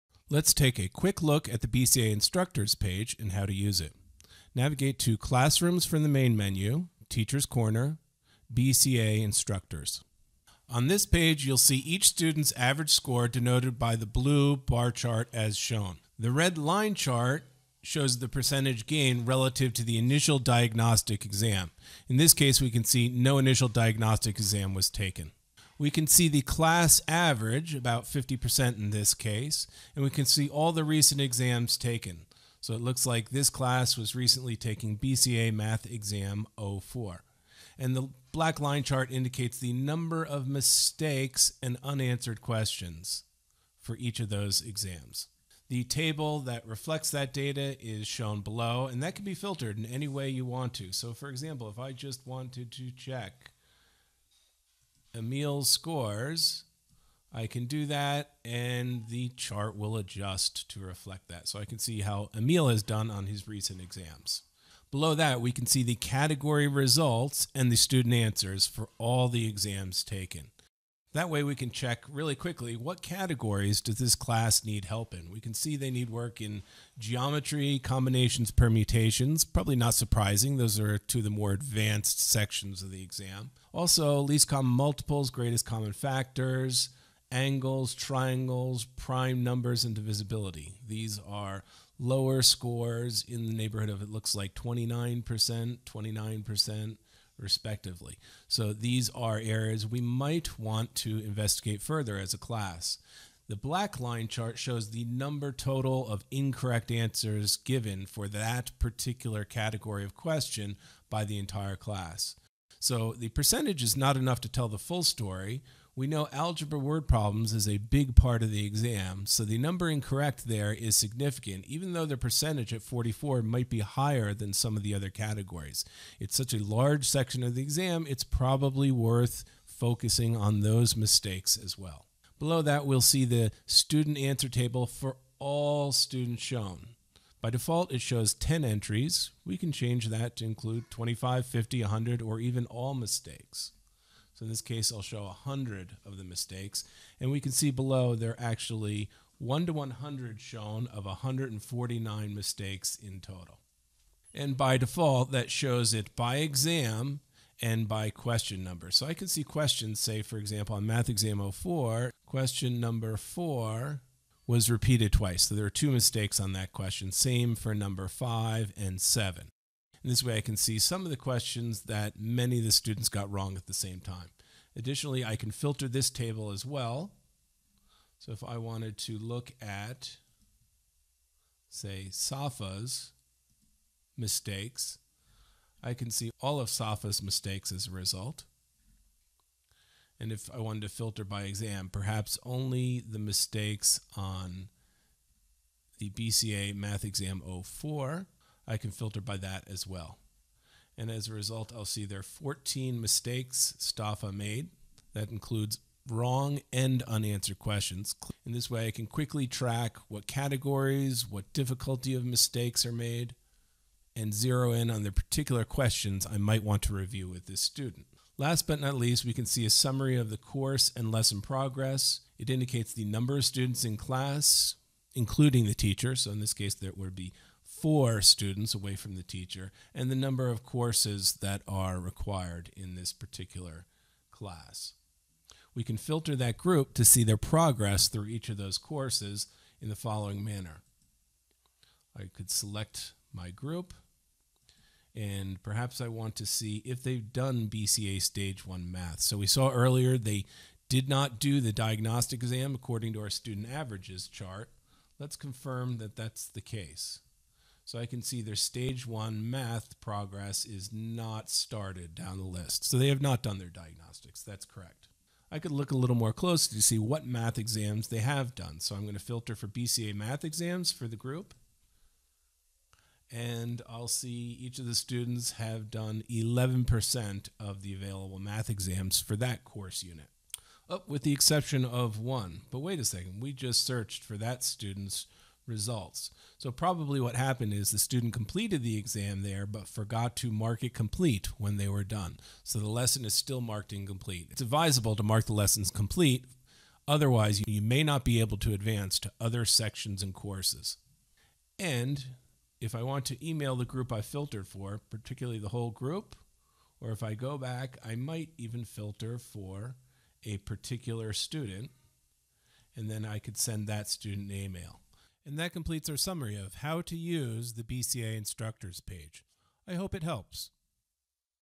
BCA Instructors Page
How to use the BCA Instructors page. Track your class scores and performance and contact students directly.